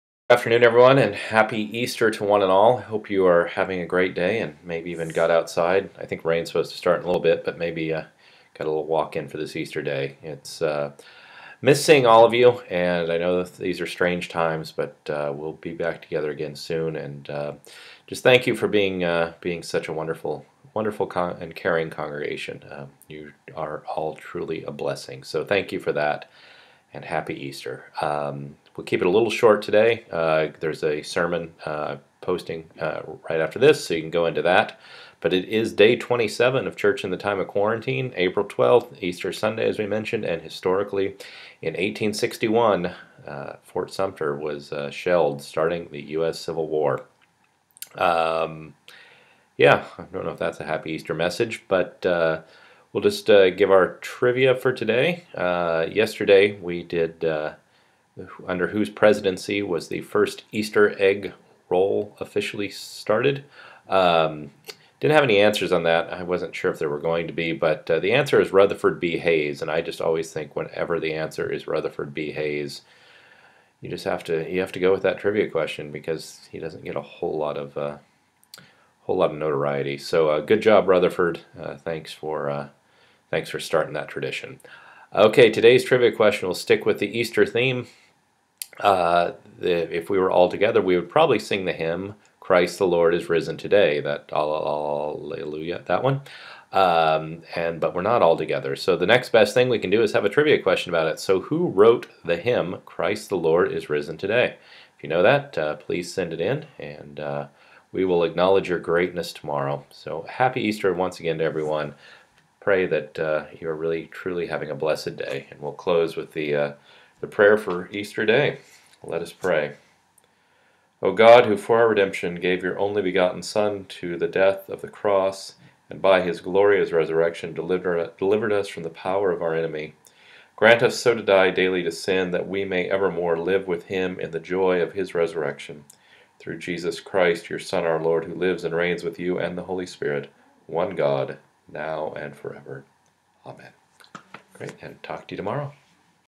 Update April 12
Episode 27: It's Easter and three cubed as well.  On this episode there's no Tyler, Taylor or Fillmore but there is a Hayes, plus Fort Sumter gets shelled and we get a little Methodist.